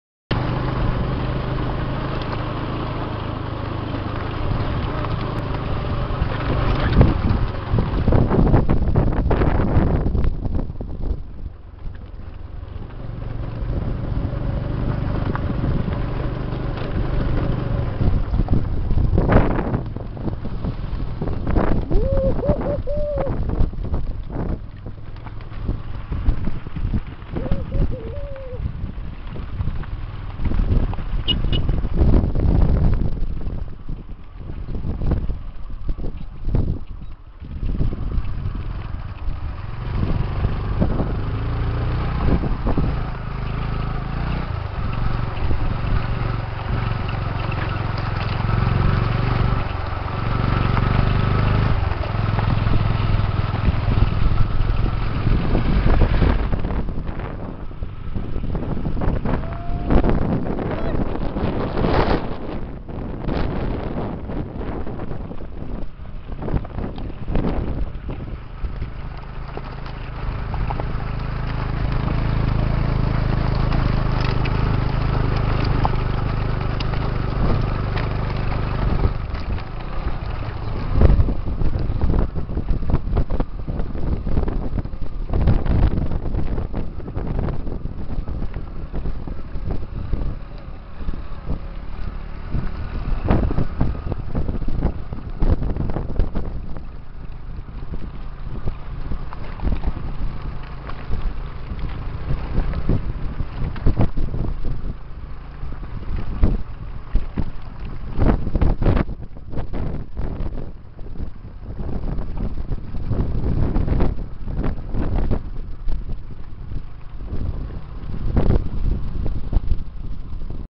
Mustang, Nepal ( in between Joomsom and Kagbeni )
On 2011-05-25, Myself KEVIN RAWAL with NRN SINGH, in between the Headquarter of Mustang(Jomsom) and Kagbeni approaching towards Muktinath tempal.